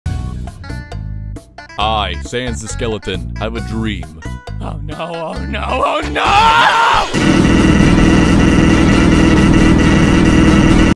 Grian has a dream. He also has explosives.
Well, Grian appears to have a end crystal, we all know where this one went don’t we travellers? No one is safe anymore. Anyways I did what I said I would and uploaded one more! See I’m trustworthy! Now sell you soul to me (I am kidding, please do not sell your soul to me.)! Anyways, auf wiedersehen travellers!